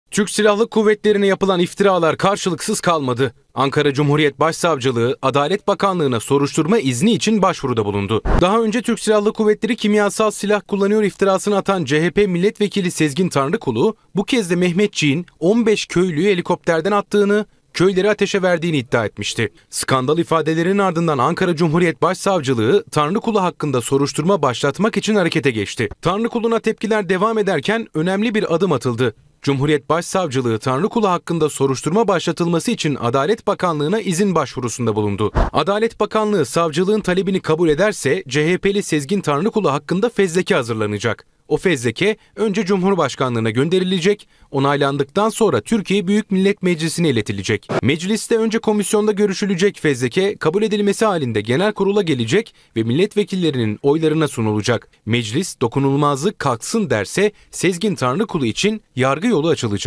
Sezgin Tanrıkulu’nun Küstah İftirası Karşılıksız Kalmadı!
TSK'ya iftira atan CHP Milletvekili Sezgin Tanrıkulu hakkında Cumhuriyet Savcılığı Adalet Bakanlığı'na soruşturma izni için başvuruda bulundu. Talep kabul edilirse Tanrıkulu’nun dokunulmazlığının kaldırılması yönünde fezleke hazırlanacak.